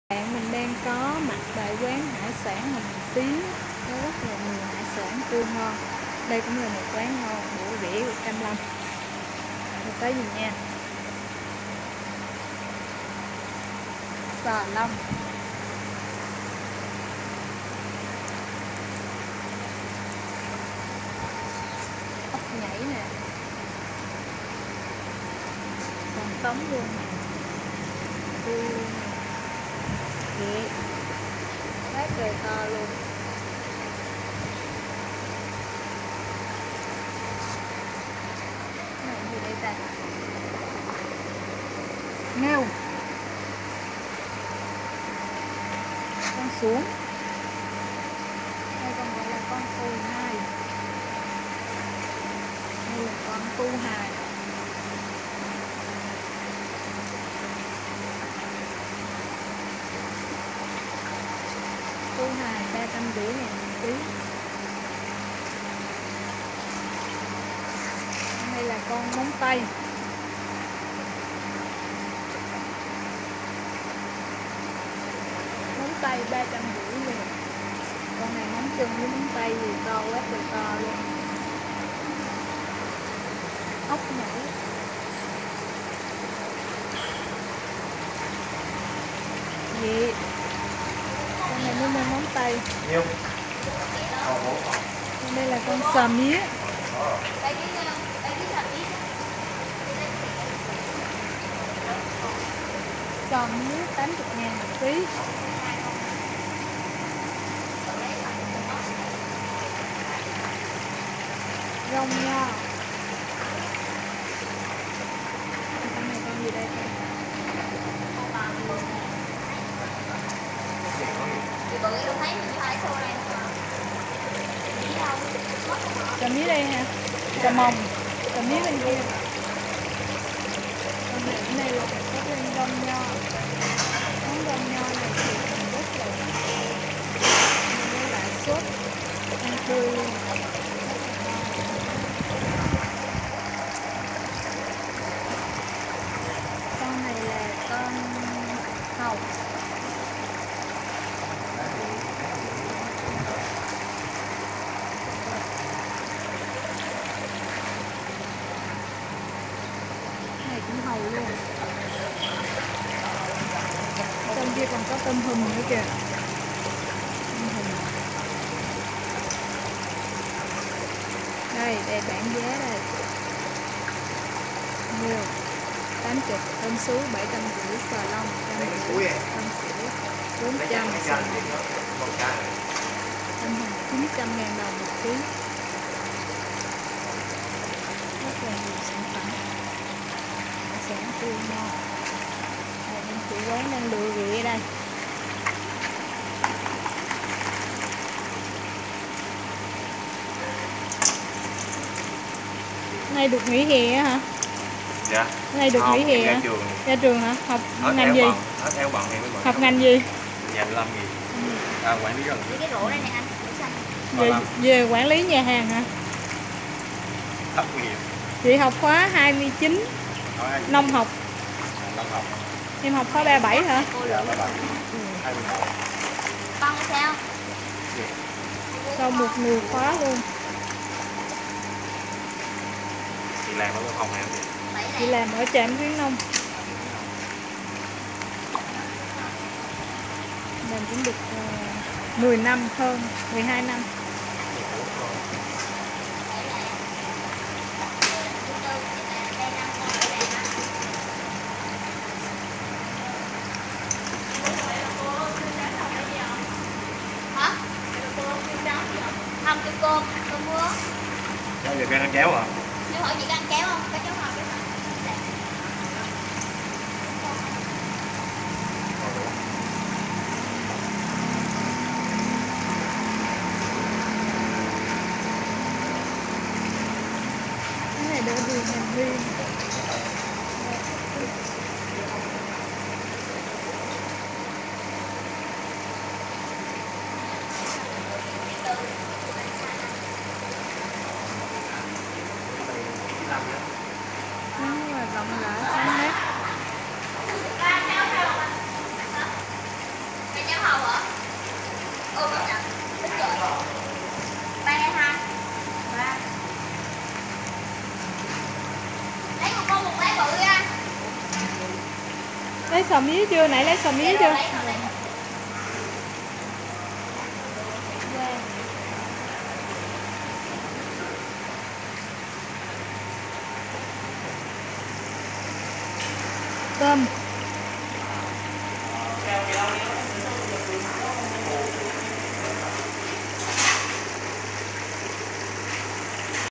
REVIEW QUÁN HẢI SẢN HỒNG TIẾN CAM LÂM
Quán hải sản Hồng Tiến cũng là một trong số quán hải sản ngon, bổ rẻ của Cam Lâm. Dù không nằm gần vị trí gần biển hay quanh Đầm Thủy Triều song quán thu hút rất nhiều thực khách sành ăn do hải sản tươi ngon, đa dạng, phục vụ nhanh chóng, đồ ăn được chế biến hợp khẩu vị và giá cả phải chăng.